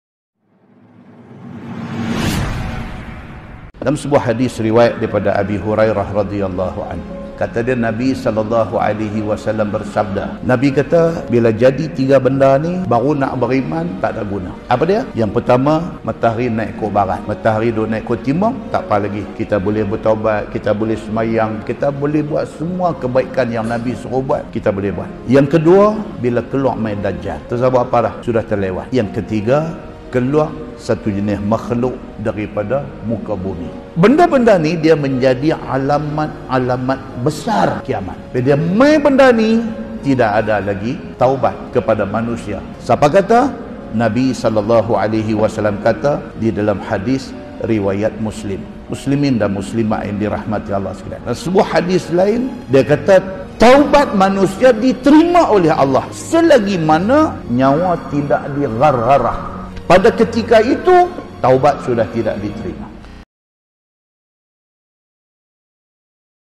Inilah tanda tanda pintu taubat telah ditutup, Ustaz Shamsuri Ahmad
Tanda tanda pintu taubat telah ditutup oleh Ustaz Shamsuri Ahmad.